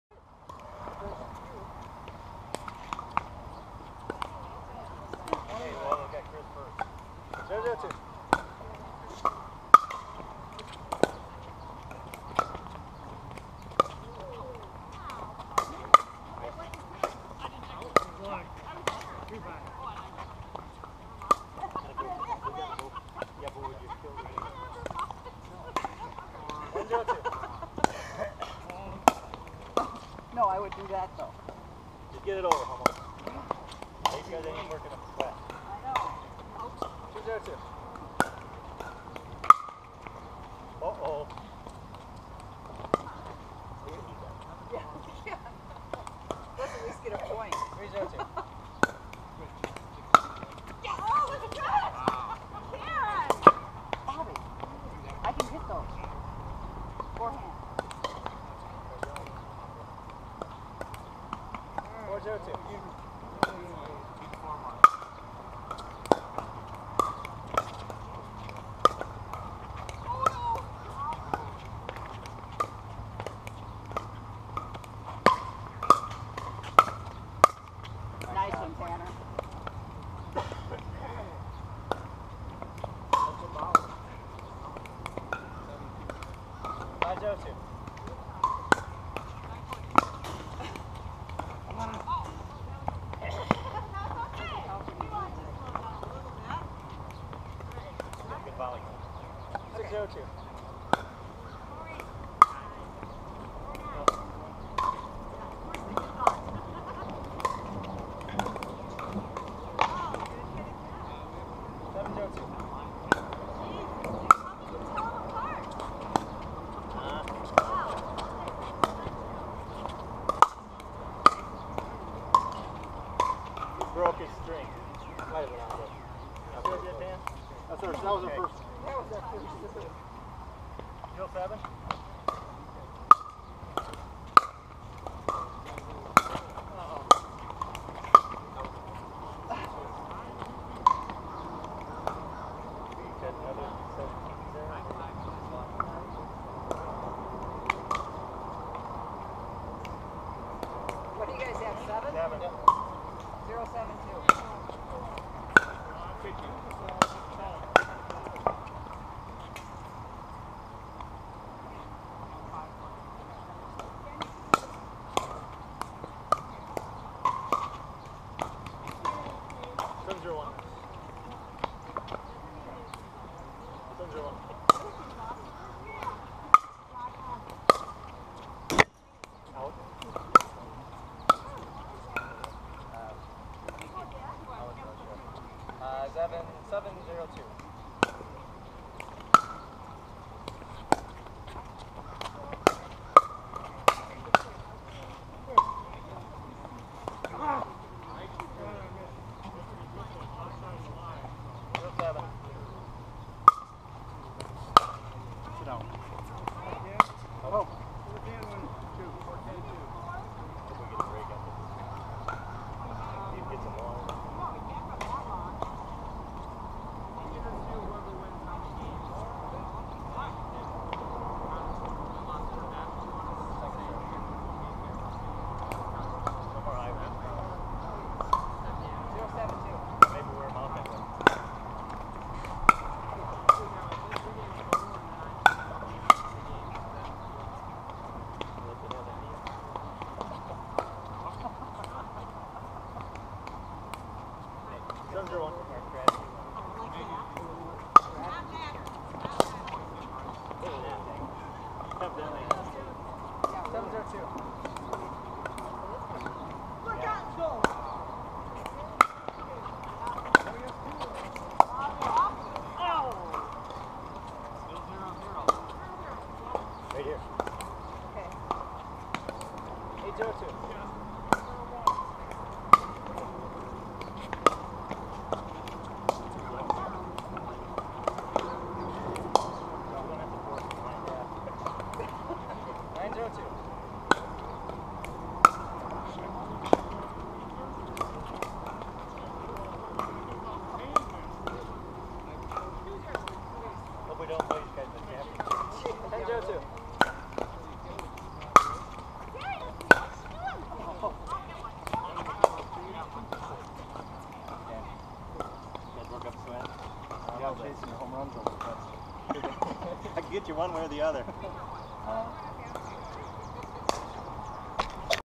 Game 1 Part 2/2 - Parks & Rec Pickleball Outdoor-Indoor Tournament #2
July 26, 2014 Saturday.  2 games to 11 win by 2.  Round robin play for seeding.  Tanner and I vs Dorita and Bob.